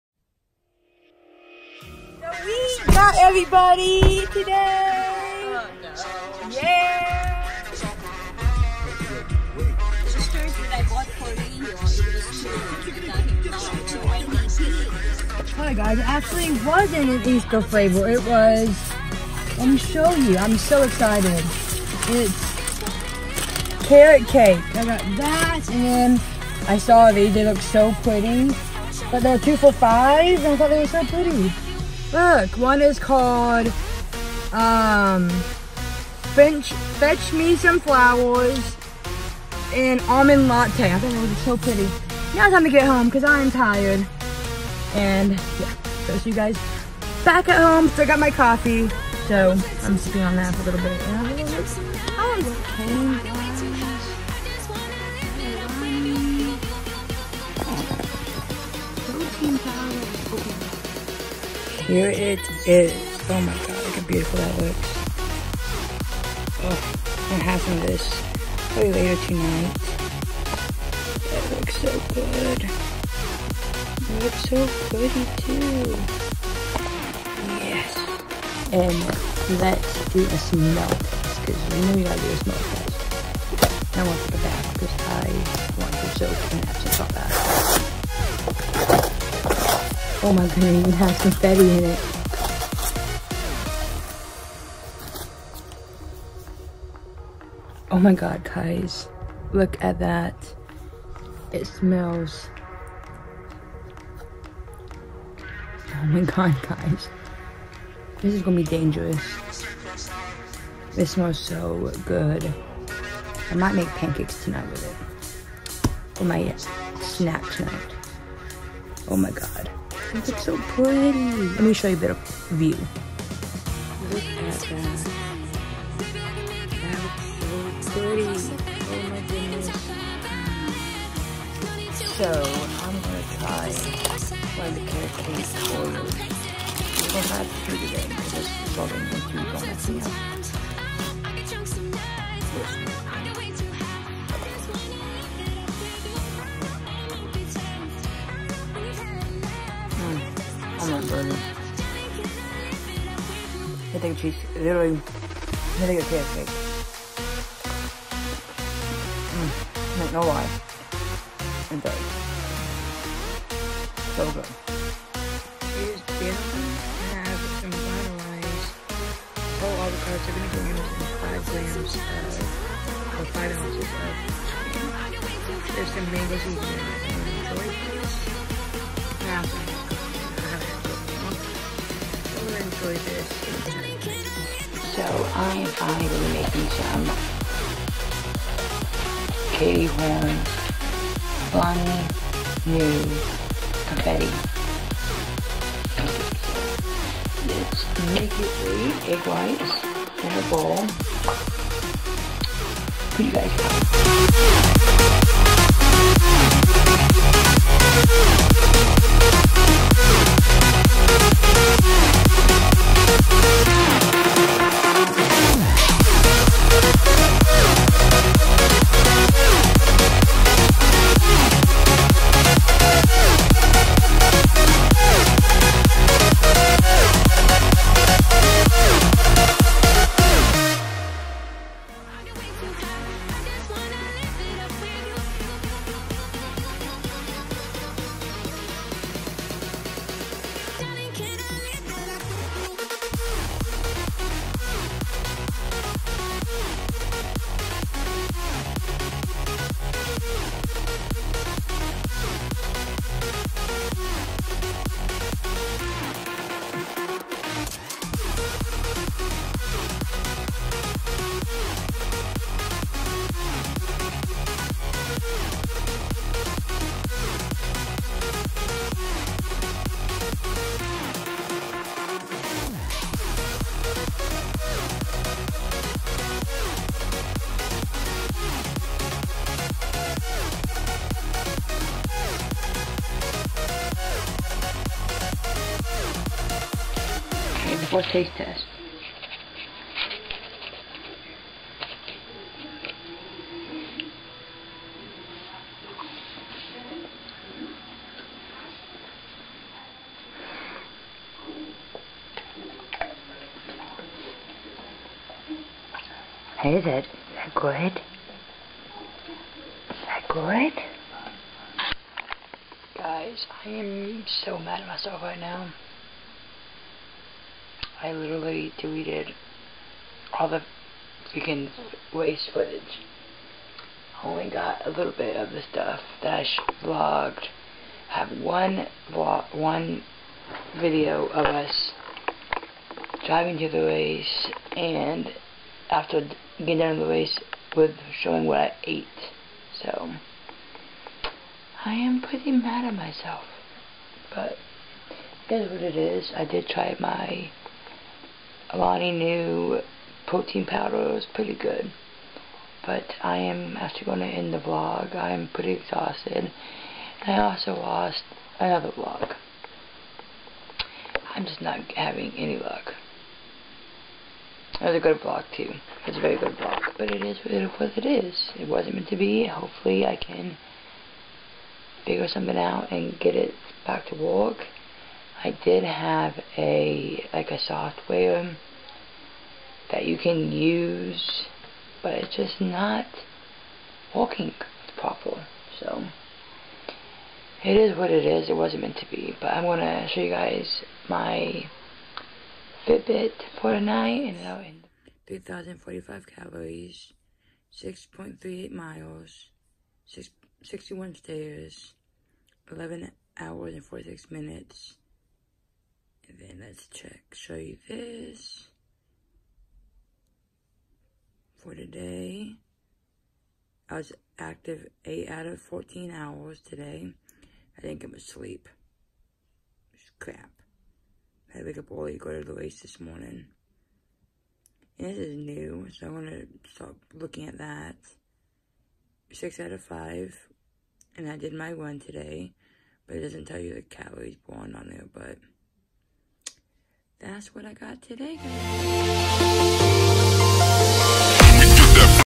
Alani Nu confetti cake Protein || Ran my first Mud Runner 🏃‍♀️ || Episode 6
Hey my lovies.
Sorry for the late post, been super busy. Did a 5k on Saturday 3.9.19 and accidentally deleted EVERYTHING. But hey that’s okay. Will be doing a rugged maniac August 17th so I will definitely be training for that one.

Also got my Alani Nu confetti cake protein and I gotta say it’s pretty lit 🔥 definitely definitely a must babes. Hope you guys enjoyed and I’ll see you in the next Vlog. Xoxo